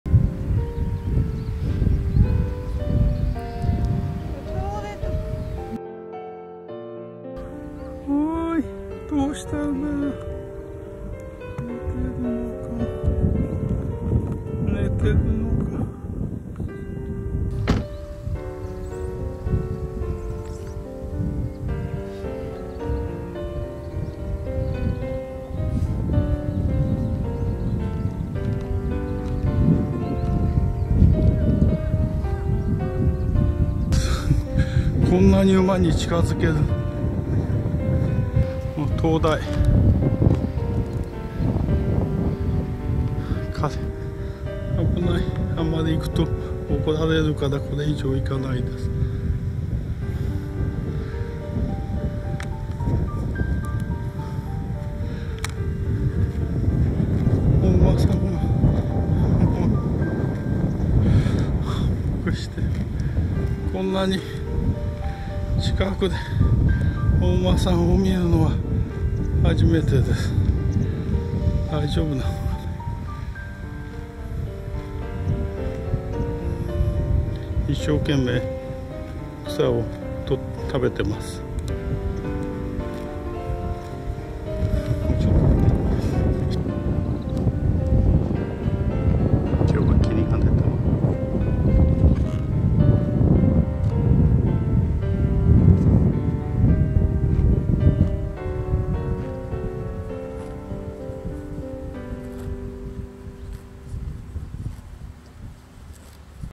青森をドライブ 親子馬に近づきました。絶景、癒しの旅、自然と街が好きです。尻屋崎
【02青森県】毎年４月～６月、仔馬が母馬に寄り添っている愛らしい光景を目にすることができる。尻屋崎周辺の牧草地には寒立馬（かんだちめ）という馬が放牧されています。青森県下北郡東通村厳しい冬にも耐えられるたくましい体格の馬である。南部馬の系統で足が短く胴が長くて、ずんぐりしている。寒立馬及びその生息地は青森県の天然記念物に指定されている。

全国ドライブの記録を編集しています。
私は江戸歴史散歩の会代表、榎本です。会のトップページ